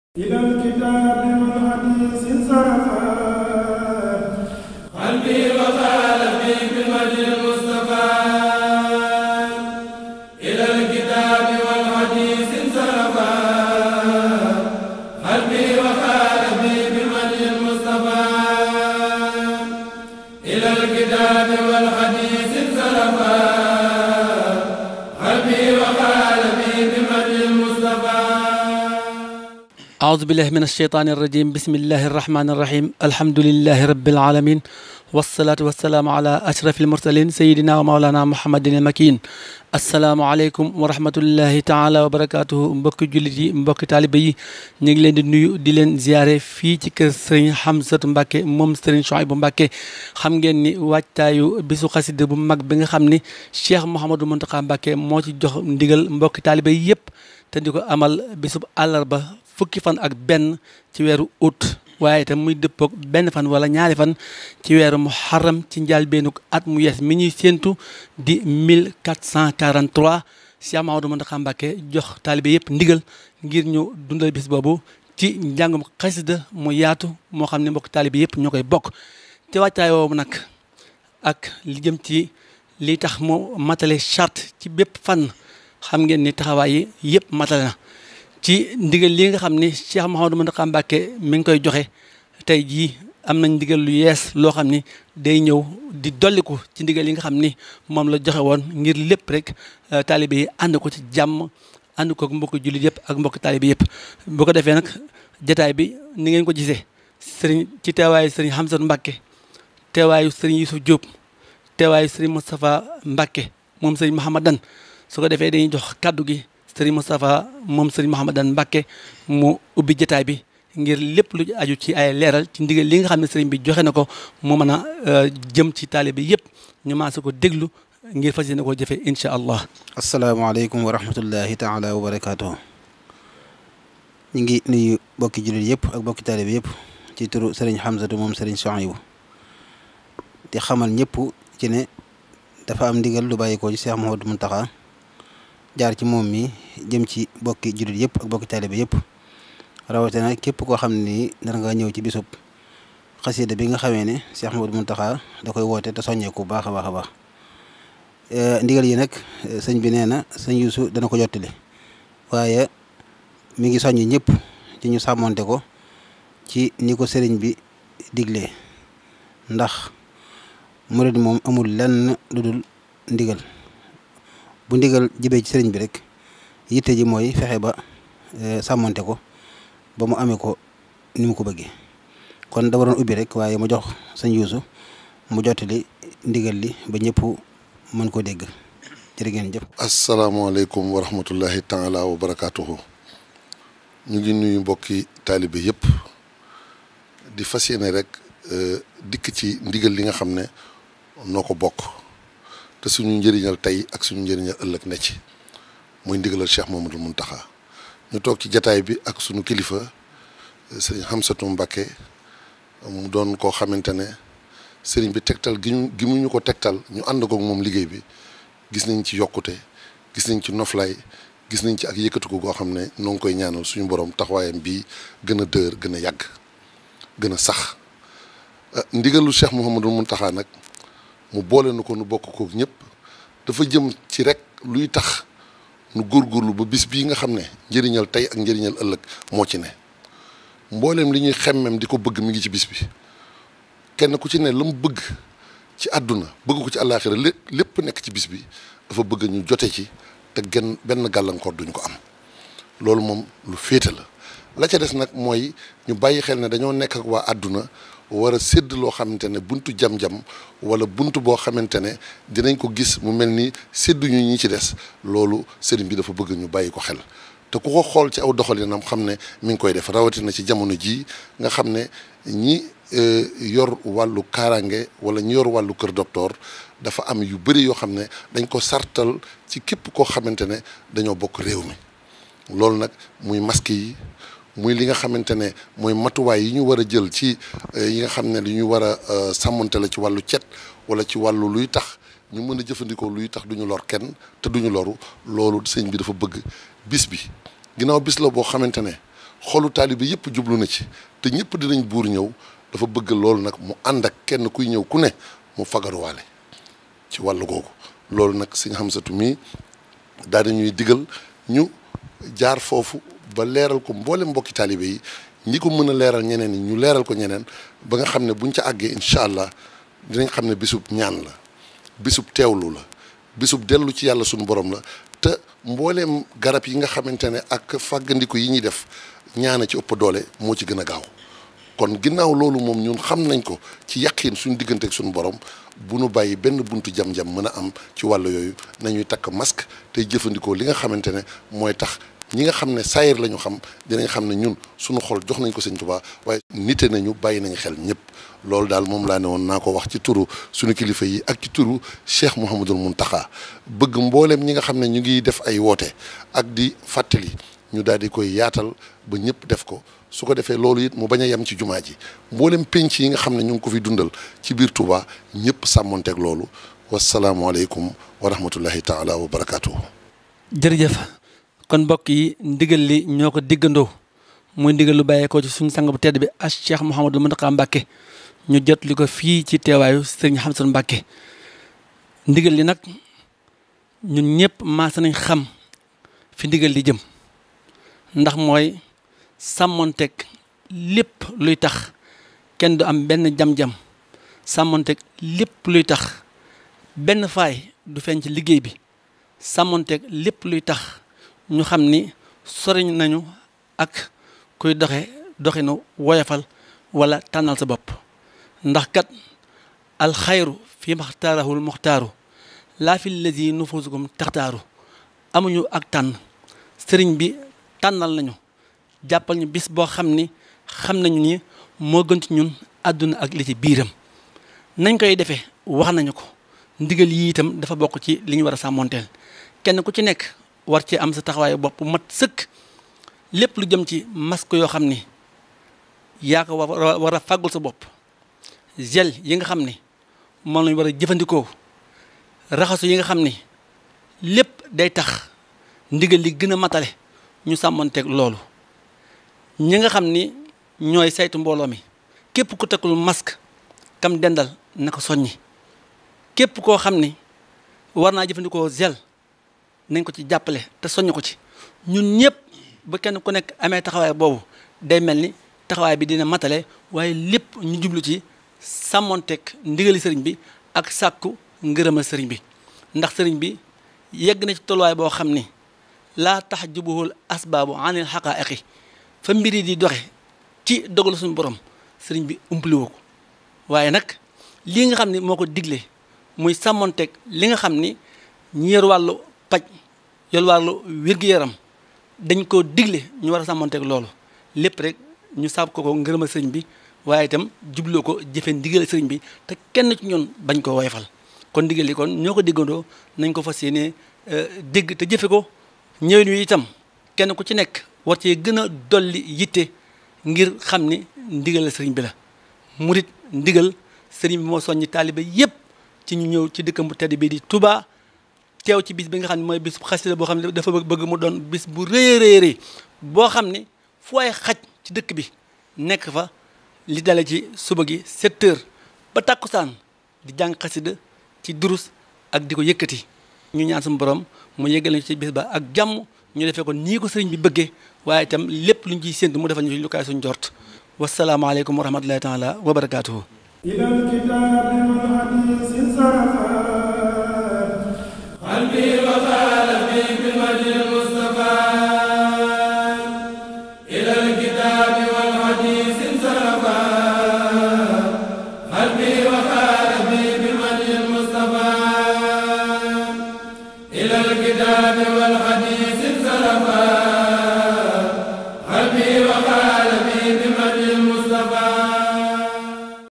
URGENT : Ndigueul du Khalif Général des Mourides pour le port de masque à la Journée Qacida
Touba MadiyanaTV